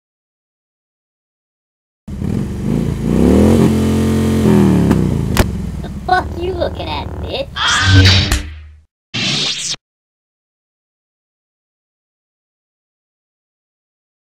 Luke vs Chibi Trooper (stop motion)
My first real animation with my SH Figuarts Luke Skywalker.